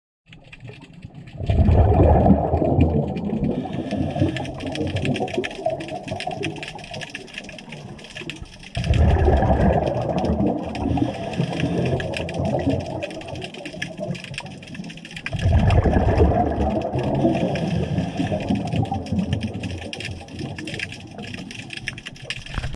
Menhaden school
I'm pretty sure these are "Gulf Menhaden" a common baitfish. It was pretty cool swimming through the school !